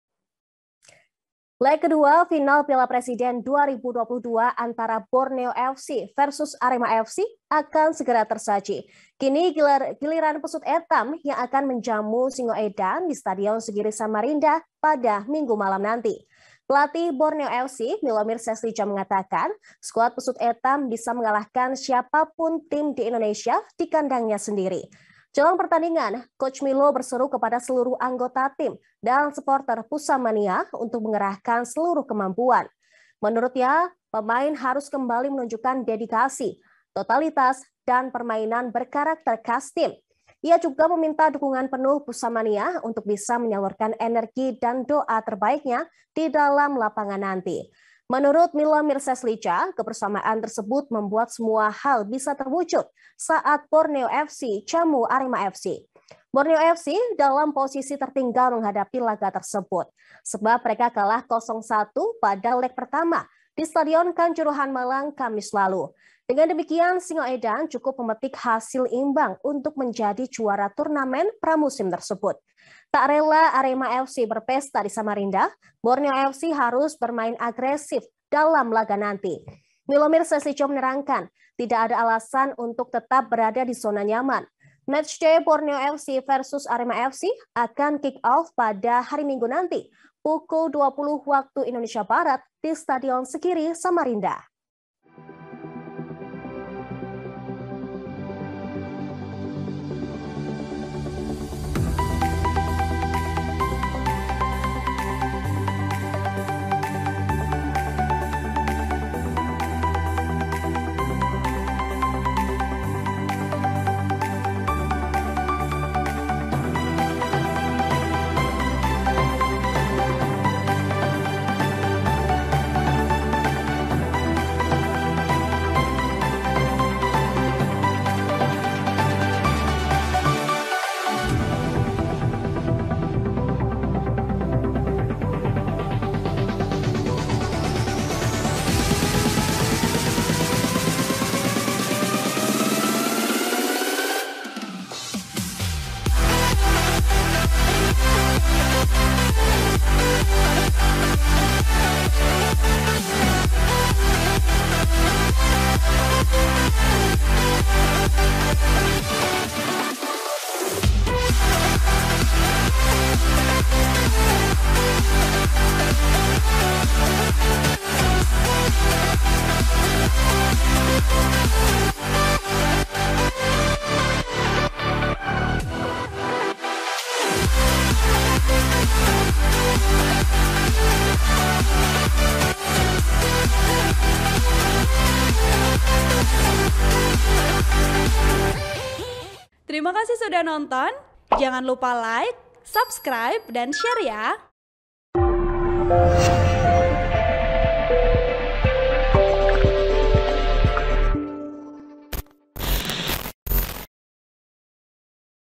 Final Piala Presiden 2022 Arema FC Vs Borneo FC, Coach Milo Sebut Pesut Etam Bisa Kalahkan Siapa Pun
TRIBUN-VIDEO.COM - Leg kedua final Piala Presiden 2022 antara Borneo FC vs Arema FC akan segera tersaji.

Kini giliran Pesut Etam yang akan menjamu Singo Edan di Stadion Segiri Samarinda, pada Minggu (17/7/2022) malam.

Pelatih Borneo FC, Milomir Seslija mengatakan skuad Pesut Etam bisa mengalahkan siapapun tim di Indonesia di kandangnya sendiri.

Jelang pertandingan, coach Milo berseru kepada seluruh anggota tim dan suporter Pusamania untuk mengerahkan seluruh kemampuan.

Menurutnya, pemain harus kembali menunjukkan dedikasi, totalitas dan permainan berkarakter khas tim.

Ia juga meminta dukungan penuh Pusamania untuk bisa menyalurkan energi dan doa terbaiknya di dalam lapangan nanti.

Menurut Milomir Seslija, kebersamaan tersebut membuat semua hal bisa terwujud saat Borneo FC jamu Arema FC.

Borneo FC dalam posisi tertinggal menghadapi laga tersebut.

Sebab, mereka kalah 0-1 pada leg pertama di Stadion Kanjuruhan, Malang, Kamis (14/7/2022).

Dengan demikian, Singo Edan, julukan Arema FC, cukup memetik hasil imbang untuk menjadi juara turnamen pramusim tersebut.

Tak rela Arema FC berpesta di Samarinda, Borneo FC harus bermain agresif dalam laga nanti.

Milomir Seslija menerangkan tidak ada alasan untuk tetap berada di zona nyaman.

Matchday Borneo FC vs Arema FC akan kick off pada Minggu hari ini pukul 20.00 WIB di Stadion Segiri, Samarinda.(tribun-video.com)